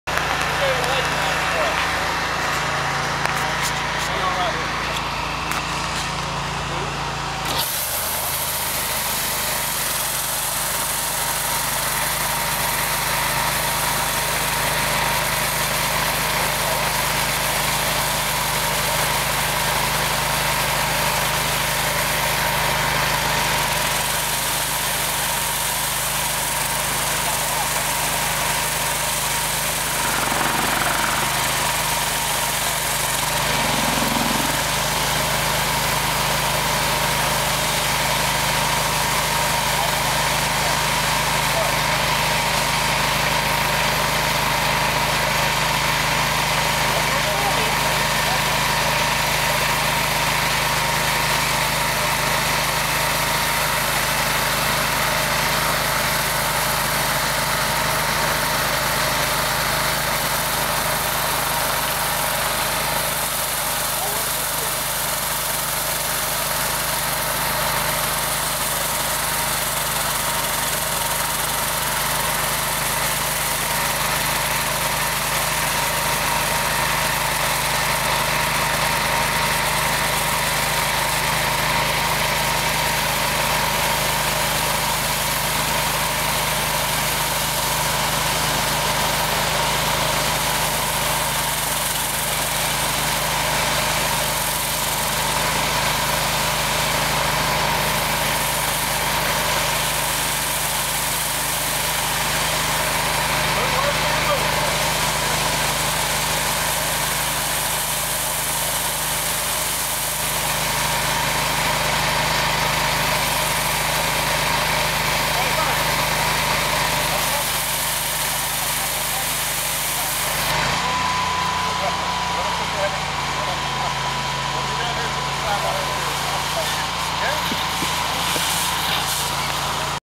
Training my Customer in Pressure washing
My customer was cleaning his driveway with a home depot pressure washer and  I was showing him how the big boys roll!